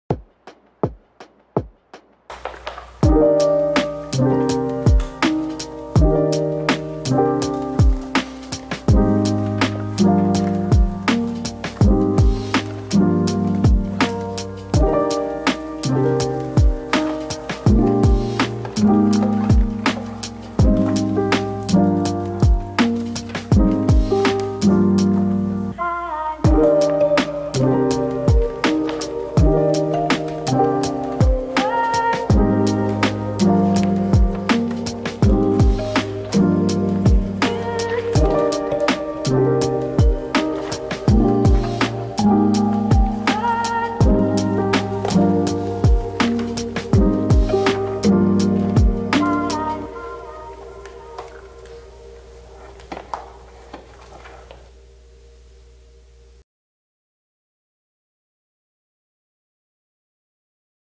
Falcon 9 Booster | Timelapse Drawing
Ik the poll wanted me to draw starship but I kinda forgot abt it lol